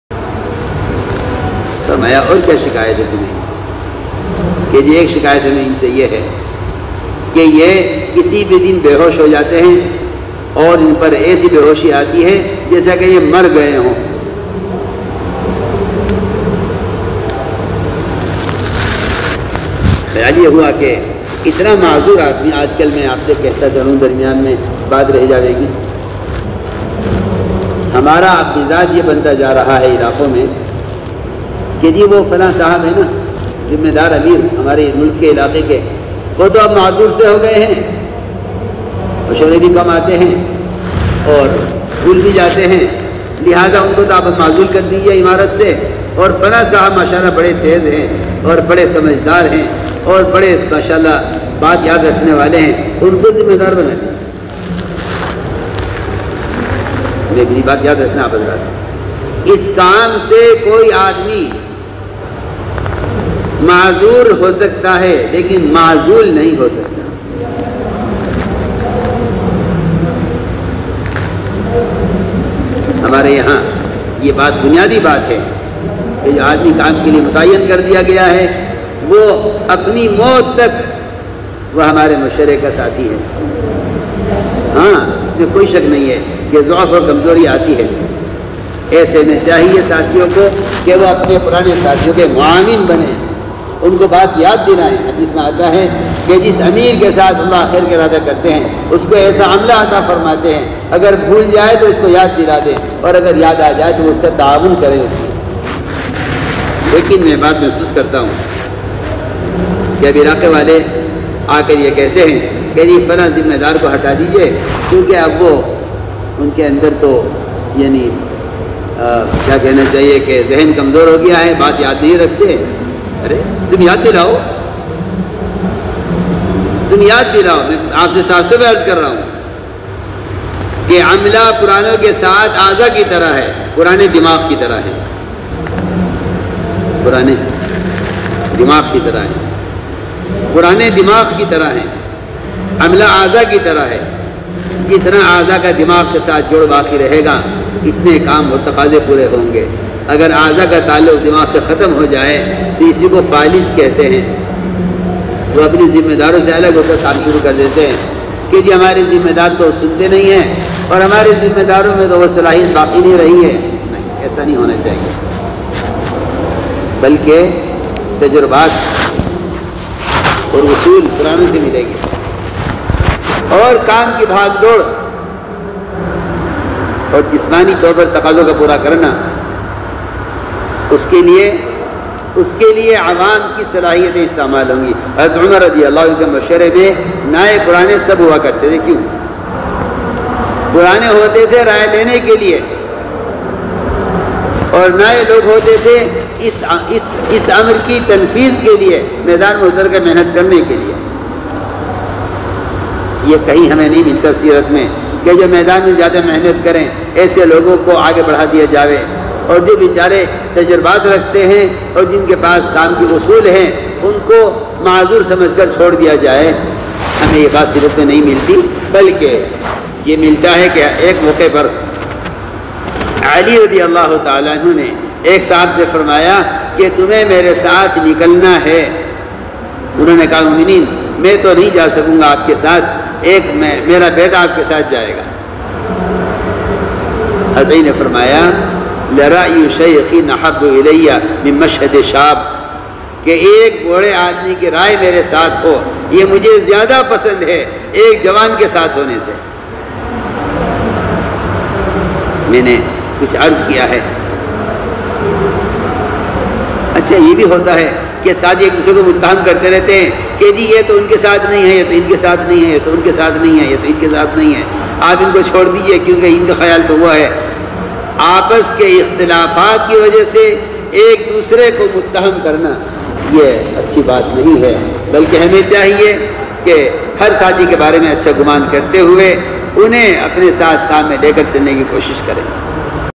Naye sathi Aaza ki Tarah hai aur purane sathi Dimag ki tarah/moulana Saad sahab 2022/tabligi Muzakra
Naye sathi Aaza ki Tarah hai aur purane sathi Dimag ki tarah

moulana Saad sahab ijtema 2022/

tabligi Muzakra

Dawat ka kaam karne wale sathiyo ke liye qimti tohfa hai

important Hidayat hazratji molana Saad sahab Db

tabligi Muzakra